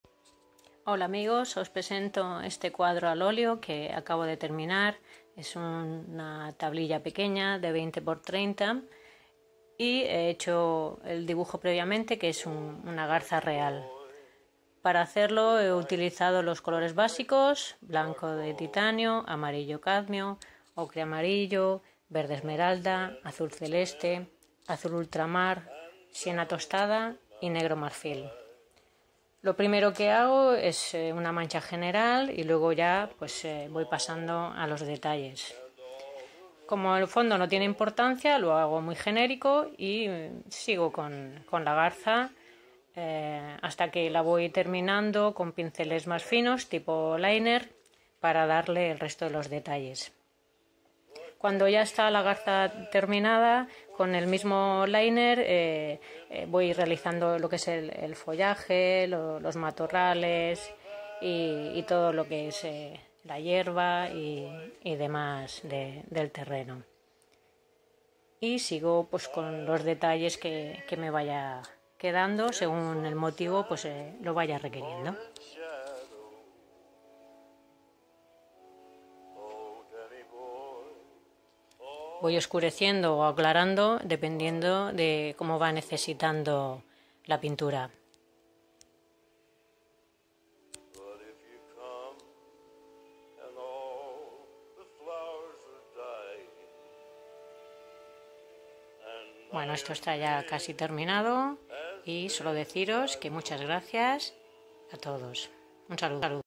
Demo al óleo"Garza Real" Alla Prima Gray Heron Oilpainting Time Lapse
Pintar una Garza Real Alla Prima al óleo Gray Heron Oilpainting Time Lapse